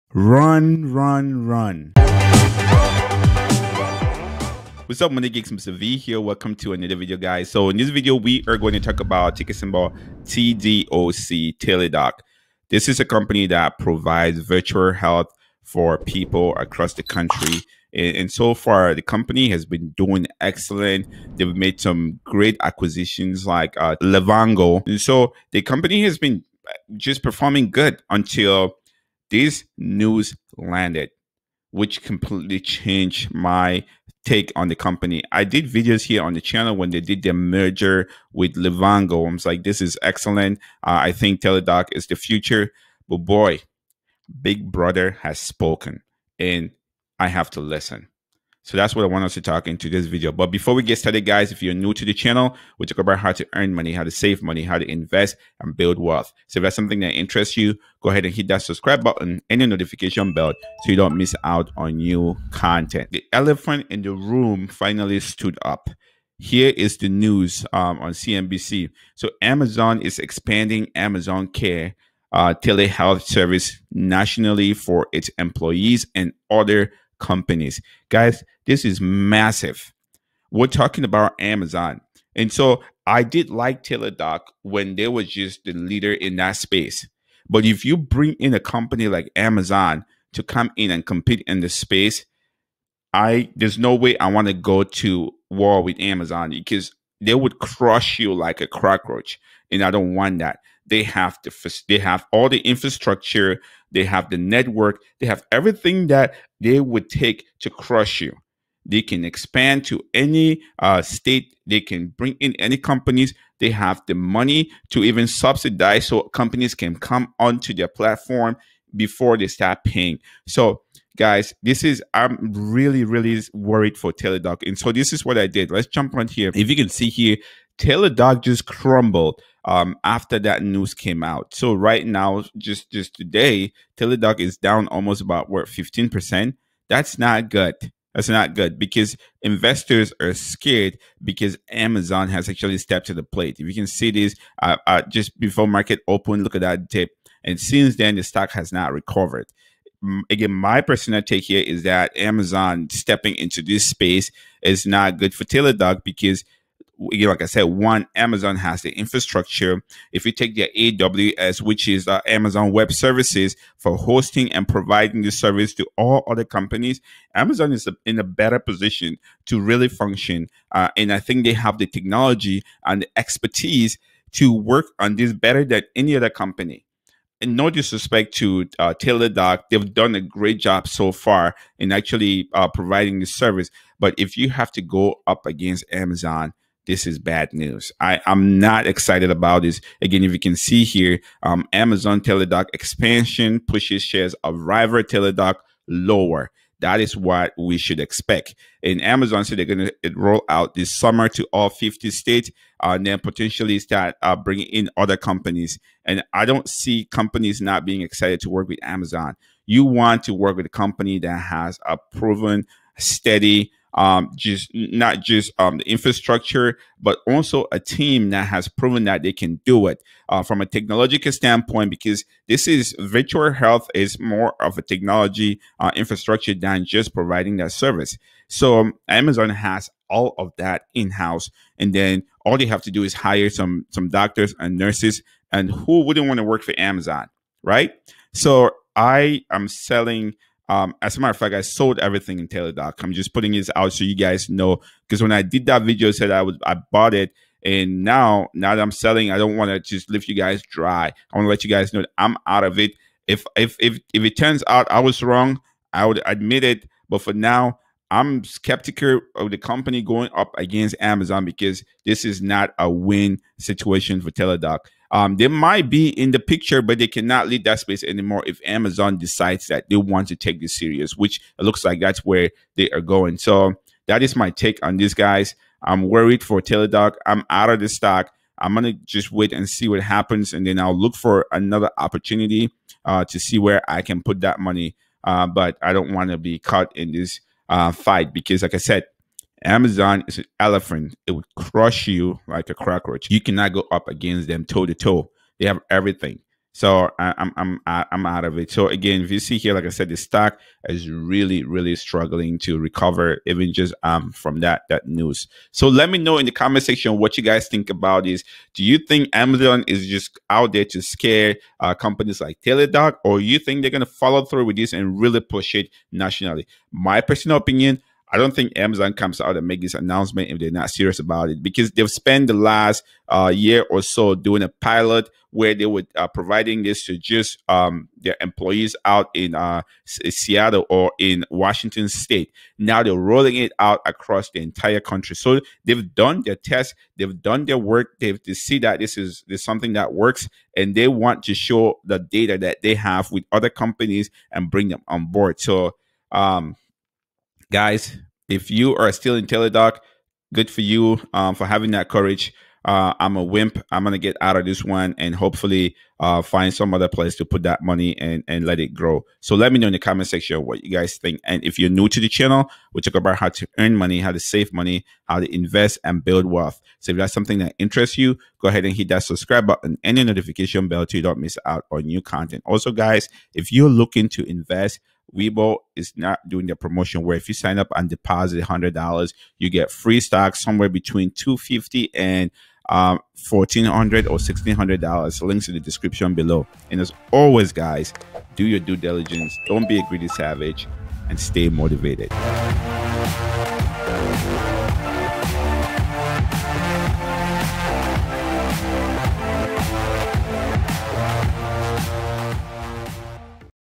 WHY I SOLD ALL MY $TDOC STOCKS?🔥🔥🔥AMAZON IS EXPANDING AMAZON CARE TELEHEALTH NATIONALLY | TELADOC
WHY I SOLD ALL MY $TDOC STOCKS?🔥🔥🔥AMAZON IS EXPANDING AMAZON CARE TELEHEALTH SERVICE NATIONALLY
Amazon will expand its virtual health pilot program, Amazon Care, to all of its U.S. employees starting this summer.
The company will also offer the virtual health program as a service for other employers nationally.
Amazon Care launched a pilot program two years ago to provide convenient urgent care visits virtually for the company’s employees in Washington state.

My Favorite Investing Book
My Favorite Personal Development Book

Based in Minneapolis
Mr. V